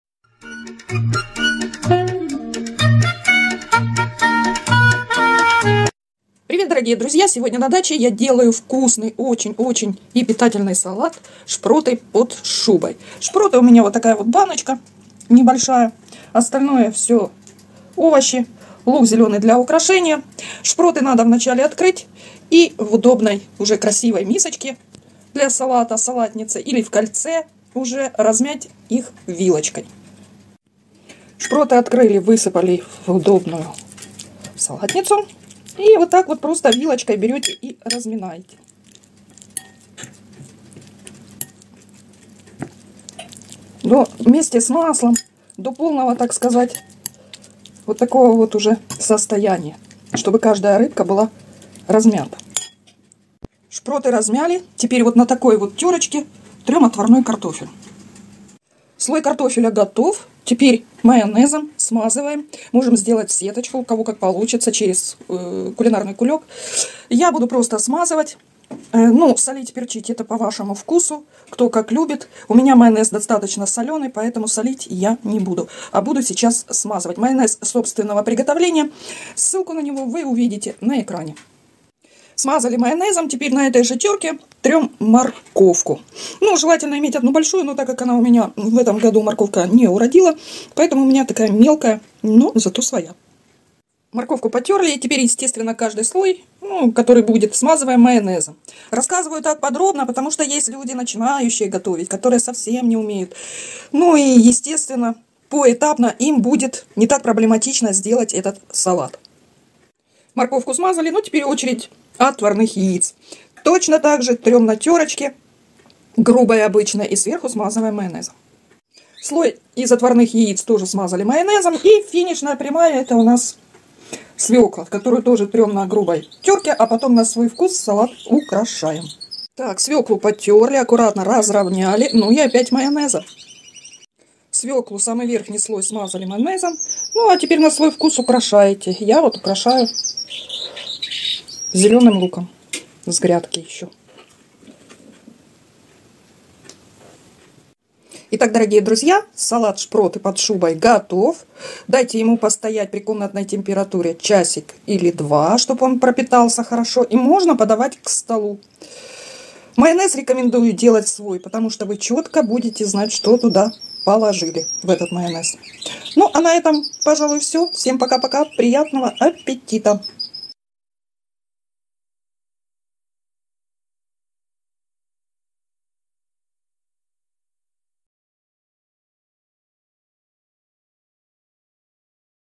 Салат - шпроты под шубой это вкусный и доступный для всех рецепт
Ингредиенты:
Шпроты - 1 банка
Картофель в мундире - 1 большая
Морковь отварная - 1 большая
Яйца отварные - 2 шт
Свекла - 1 шт
Майонез - количество по вкусу